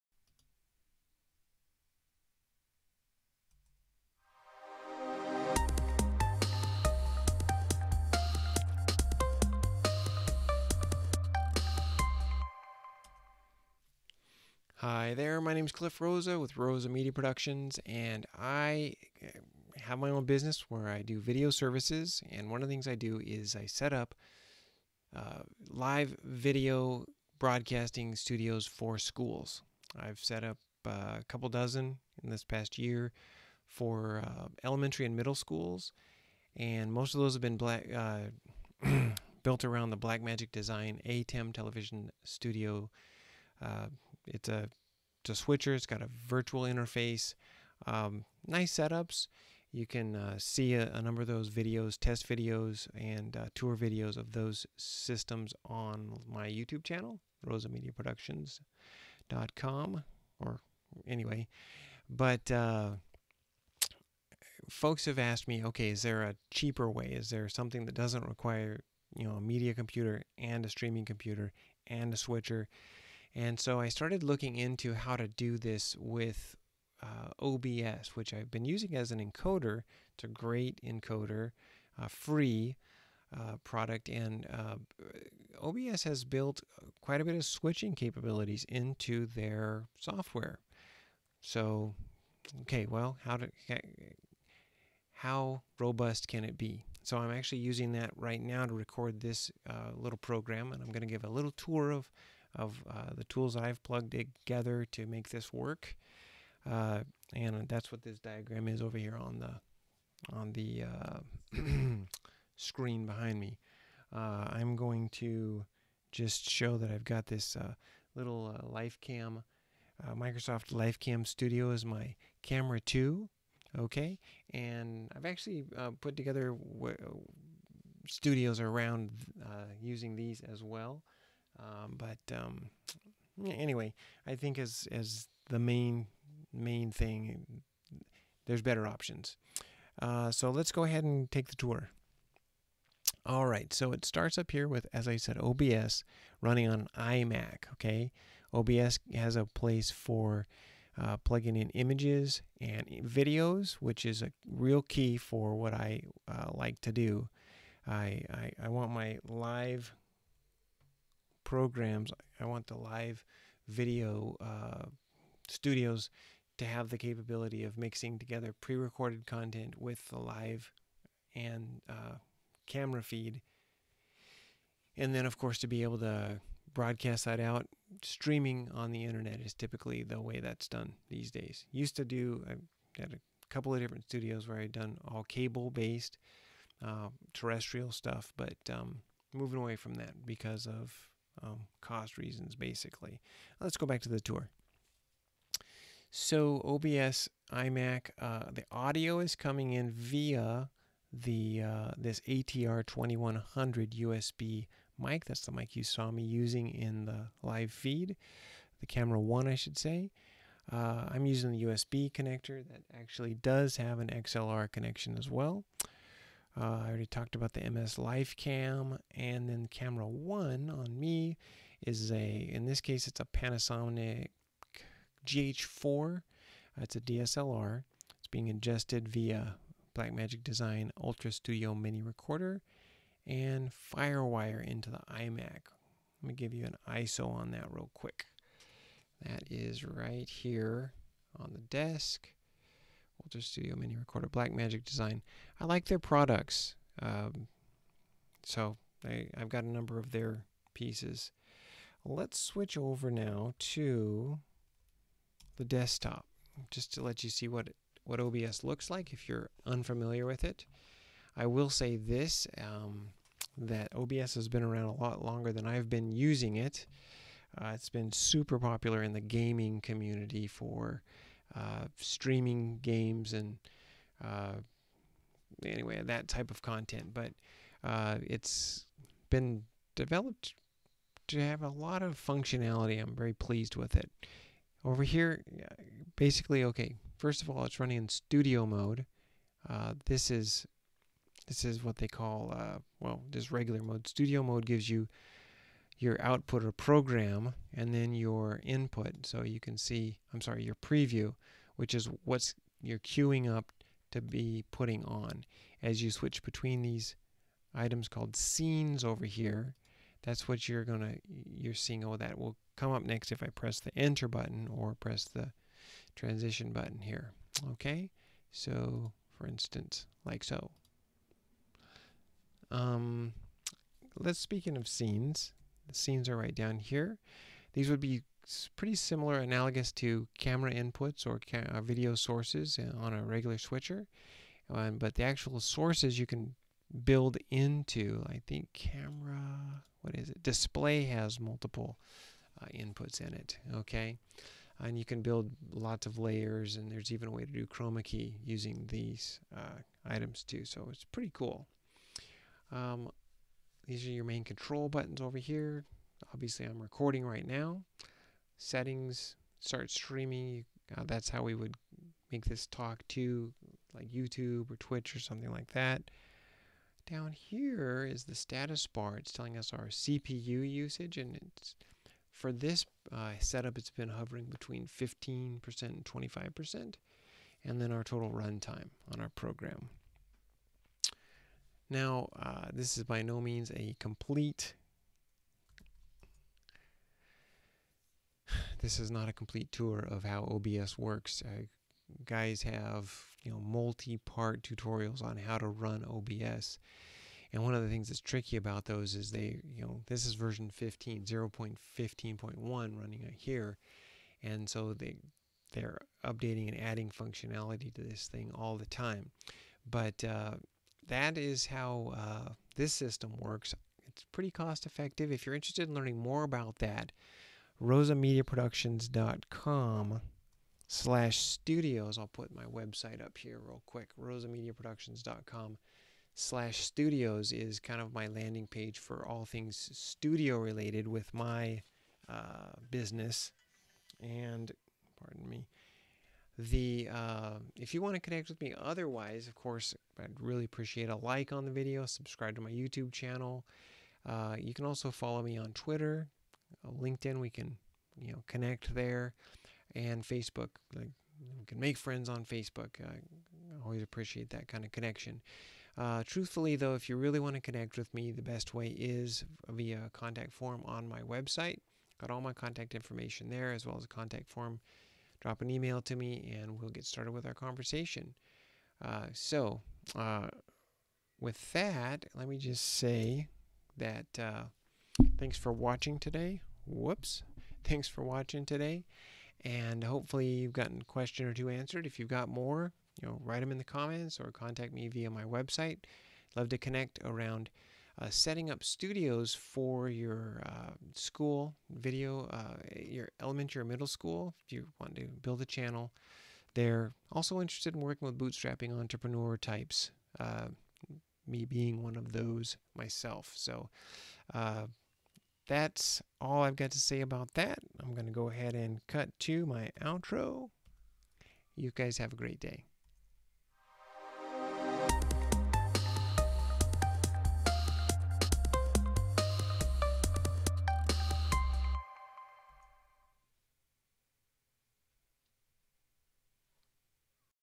Cost Effective 2 Camera Live Production Video Broadcast
My name is Cliff Rosa with Rosa Media Productions. I have my own business where I do video services. One of the things I do, is setup live video broadcasting studios for schools. I've setup a couple dozens in this past year for elementary and middle schools. Most of those have been built around the Blackmagic Design ATEM Television Studio. It's a switcher, it's got a virtual interface. You can see a number of those videos, test videos, and tour videos of those systems on my Youtube channel, Folks have asked me, "Is there a cheaper way? Is there something that doesn't require a media computer, a streaming computer, and a switcher?" So that is why I created this video using a cost effective 2 camera live production video broadcast system.

I started looking into how to do this with OBS. Which I've been using as an encoder. It's a great free encoder. OBS has built quite a bit of switching capabilities into their software. How robust can it be? I use OBS to record this little program. I show it in the diagram on the board in the video.  I use a Microsoft Lifecam Studio as my handheld camera 2. I've actually put together studios using these as well. I think as the main camera, there are better options.

Audio-Technica ATR2100-USB Cardioid Dynamic USB/XLR Microphone

Microsoft LifeCam Studio 1080p HD

Panasonic DMC-GH4 DSLR Camera

Blackmagic Design UltraStudio Mini Recorder - Thunderbolt
by Blackmagic Design

Blackmagic Design ATEM Television Studio Production Switcher

That is how the cost effective 2 camera live production video broadcast system works. It's pretty cost effective. If you're interested in learning more about this